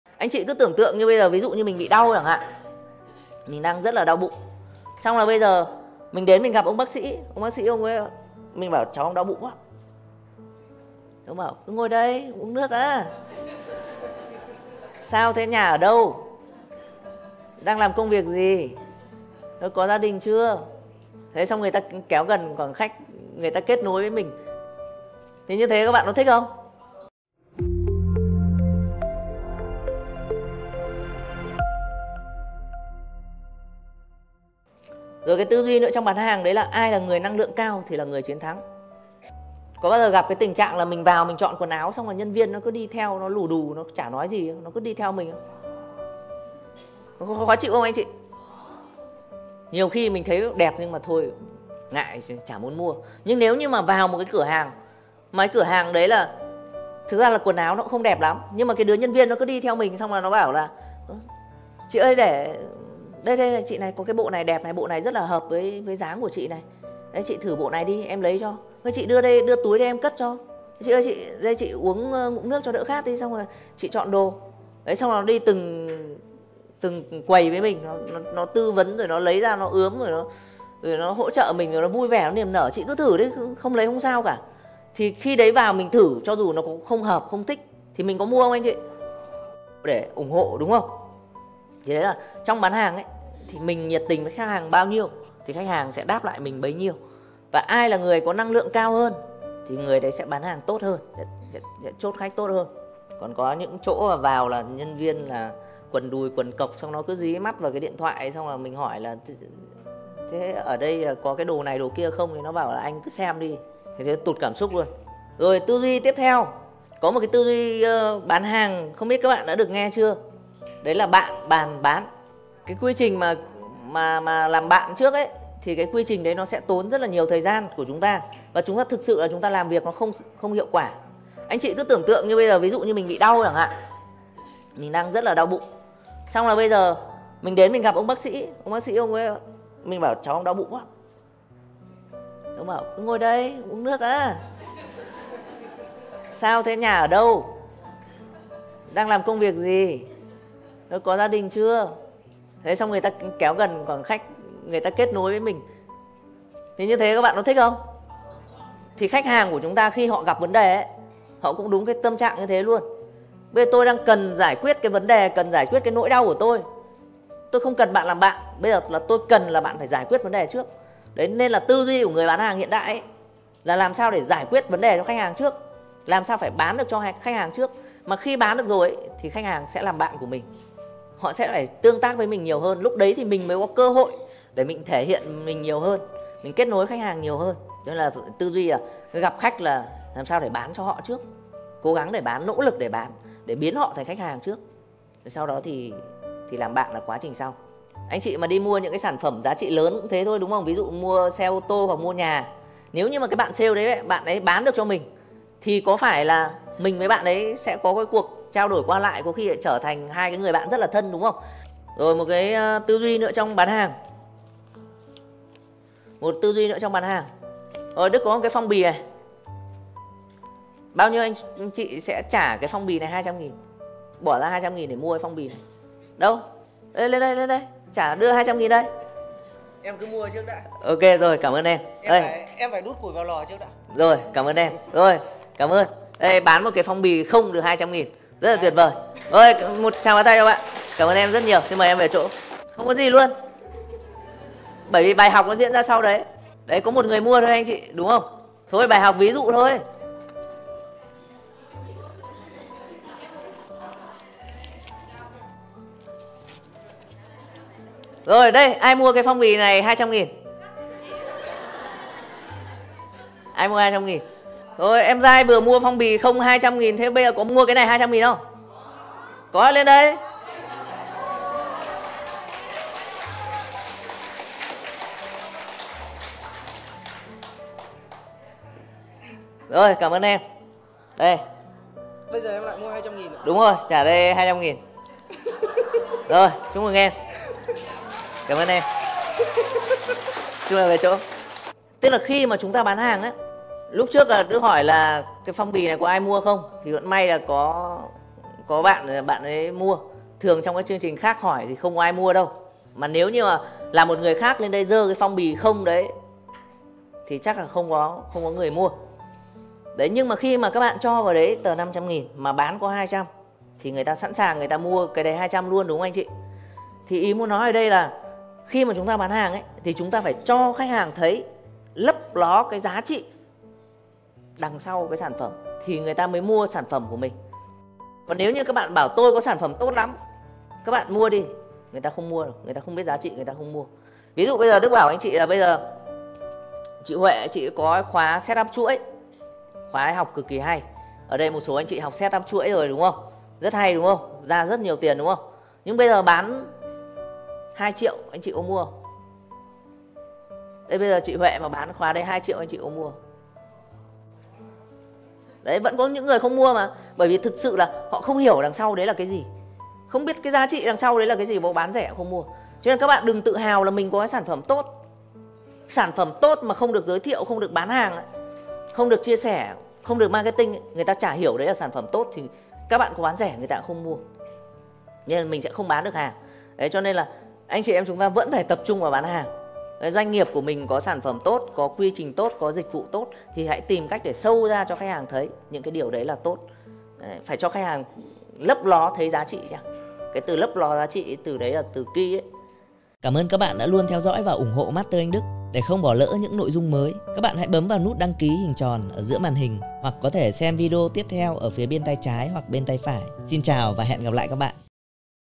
Bán bất cứ thứ gì cho bất kỳ ai nếu biết nguyên tắc này | Master Anh Đức
Bạn đã bao giờ tự hỏi làm thế nào để có thể bán được bất cứ sản phẩm nào cho bất kỳ ai không? Trong video này, Đức sẽ chia sẻ nguyên tắc quan trọng mà nếu bạn áp dụng đúng cách, bạn có thể thành công trong việc bán hàng cho mọi đối tượng.

Từ cách xây dựng một thông điệp hấp dẫn đến cách tiếp cận mọi đối tượng khách hàng, video này sẽ cung cấp cho bạn những kiến thức cần thiết để trở thành một người bán hàng thành công.

Và còn rất nhiều video dành cho ai muốn phát triển bản thân, kỹ năng bán hàng đừng bỏ lỡ video dưới đây: